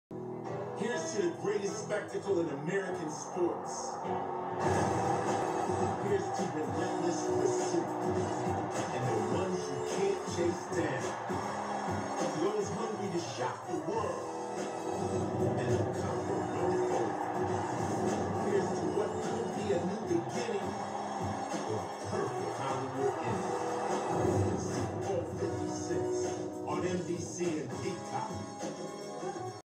Super Bowl LVI/56 2022 Promo (:30)
Man, I haven't updated/routinely checked these playlists in MONTHS.

I can't even remember what TV station/entity this was a promo for, but this is reuploaded, as you probably can guess, for the sake of my playlist.

No, that's not me standing in front of the screen with my phone. That was the work of whoever uploaded this first, and I'm glad I nabbed this before they got terminated.